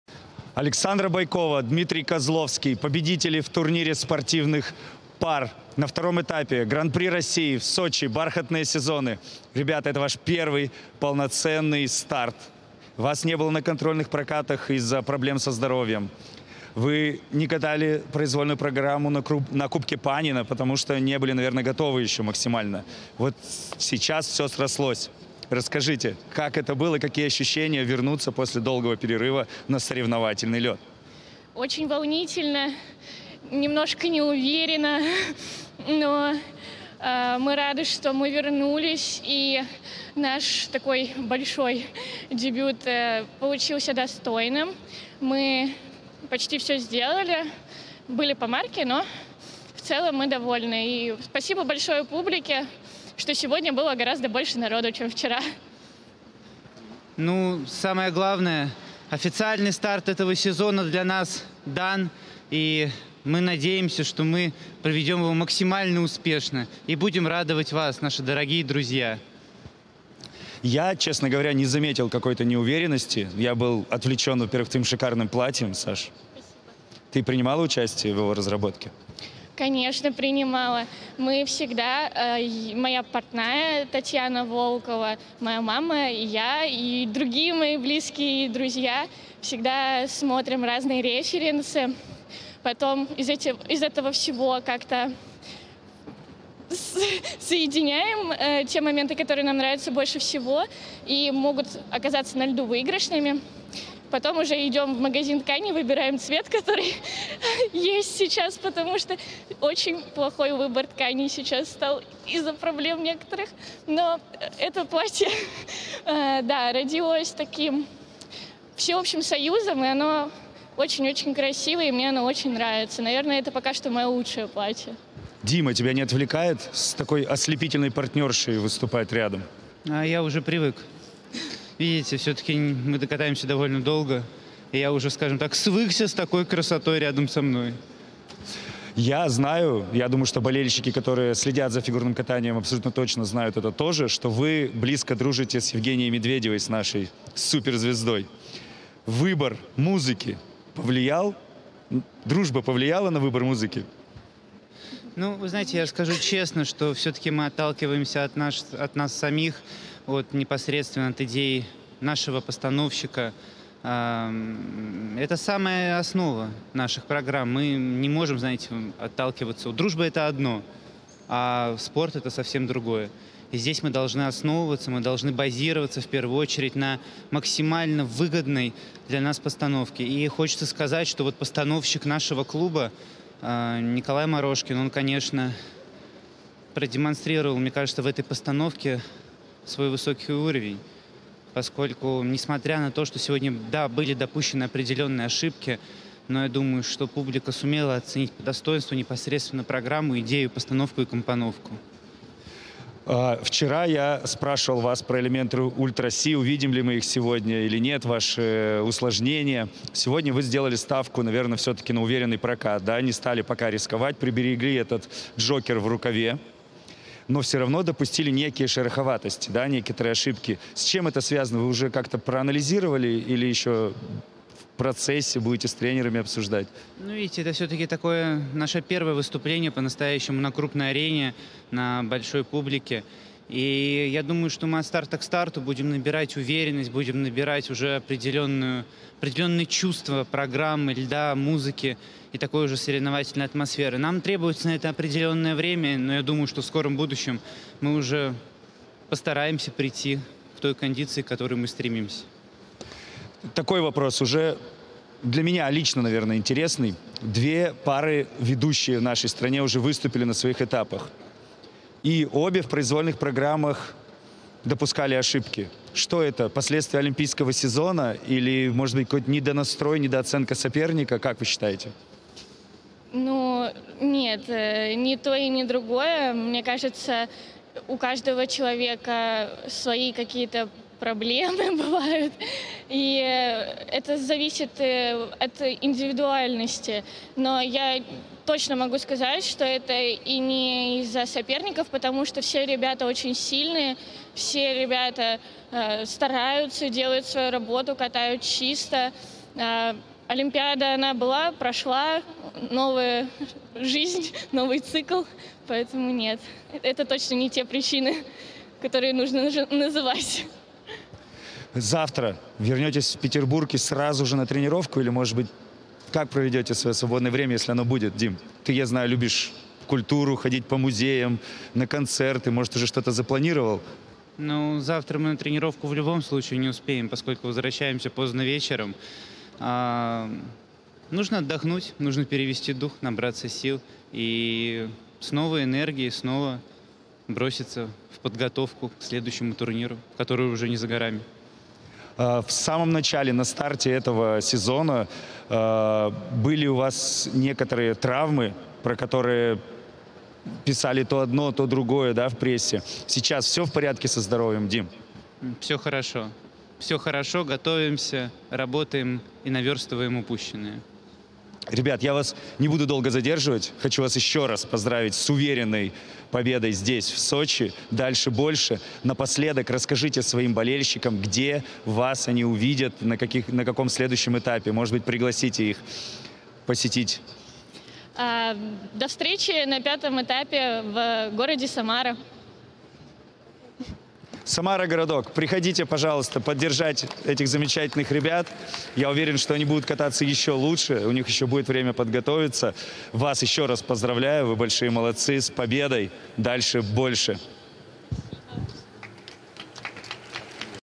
"Я уже свыкся с такой красотой рядом со мной". Александра Бойкова и Дмитрий Козловский
Победители этапа Александра Бойкова и Дмитрий Козловский рассказали о подготовке костюмов, дружбе с Евгенией Медведевой и личных приоритетах в выборе программ — это и многое другое в интервью Максиму Транькову.